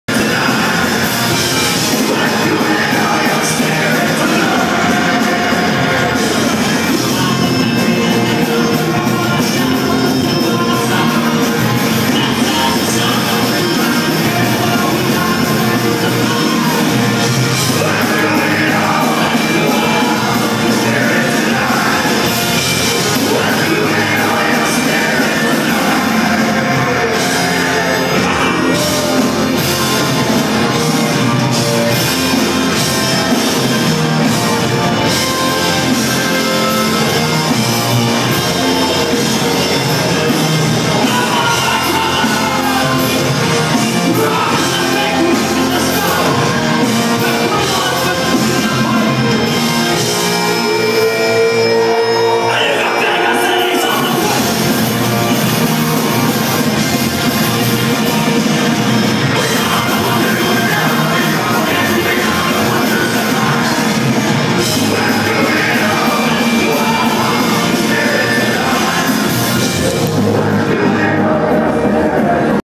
Dropkick Murphy Raleigh 6/4/13
Another minute of moshing, featuring a giant human.